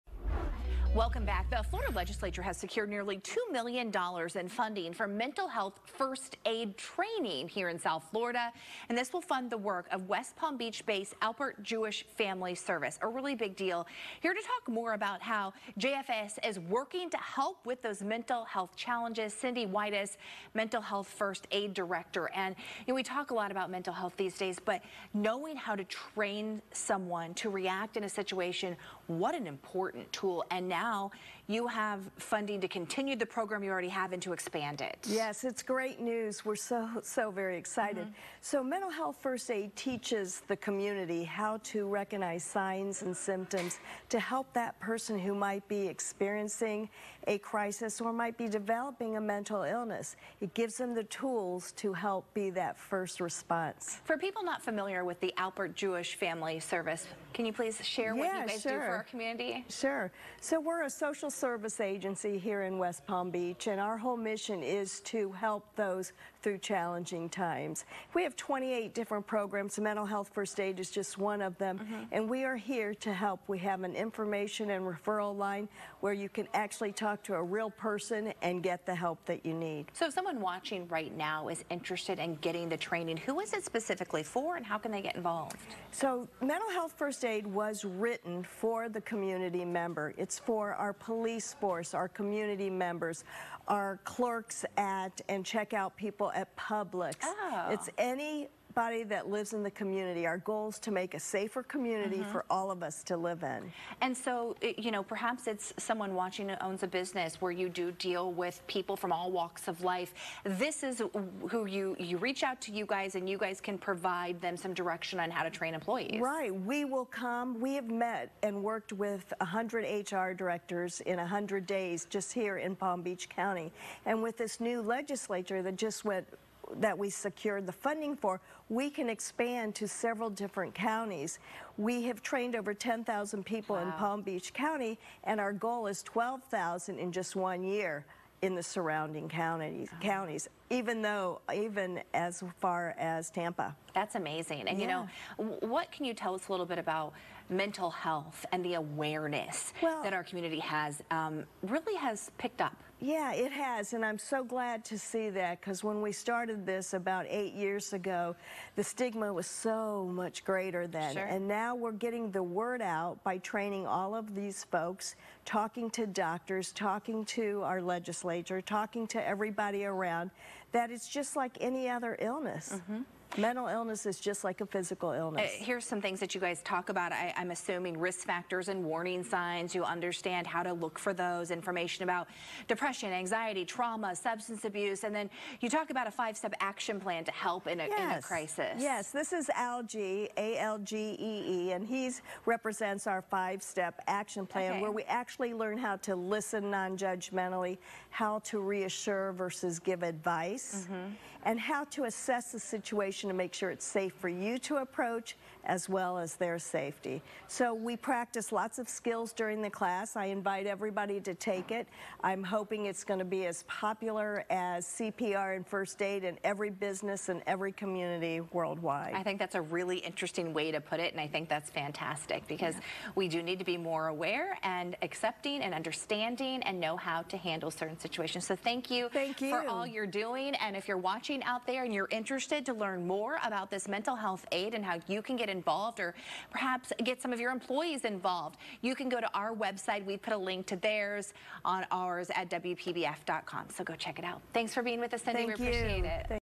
WPBF ABC 25: Cindy Wides Talks Mental Health First Aid
Cindy Wides talks about Mental Health First Aid with WPBF ABC 25.